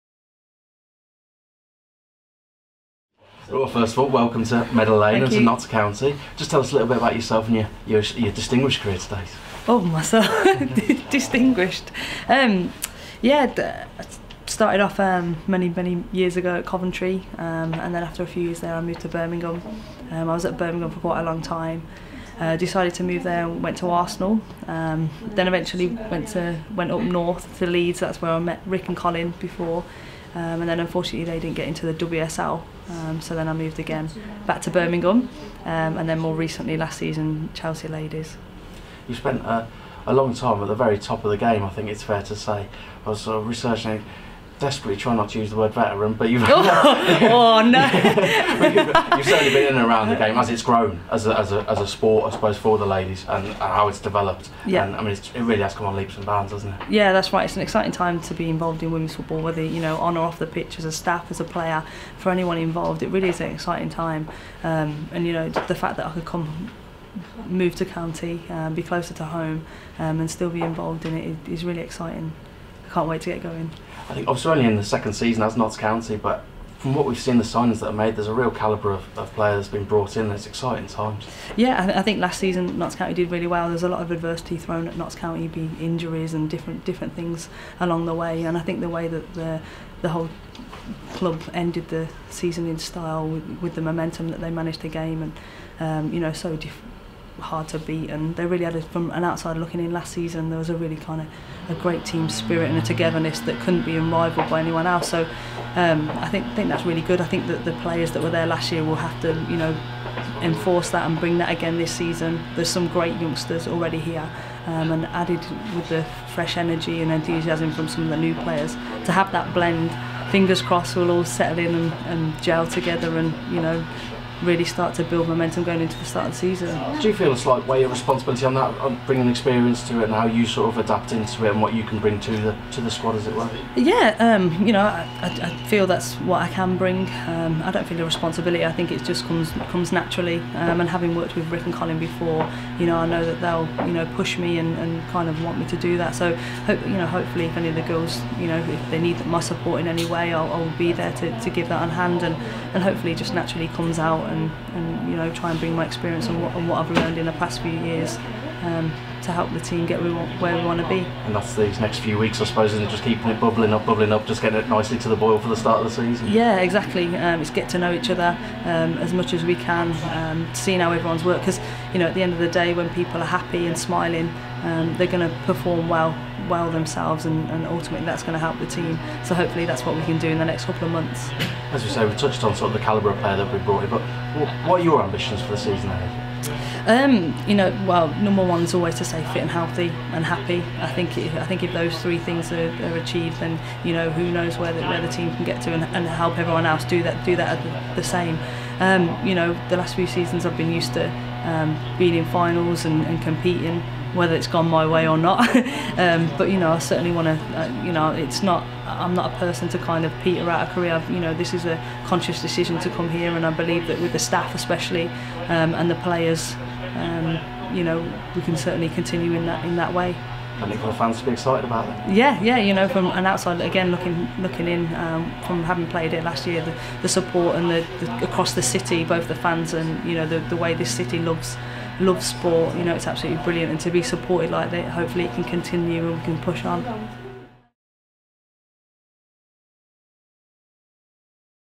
Laura Bassett
Laura Bassett speaks to the media after signing for Notts County Ladies FC.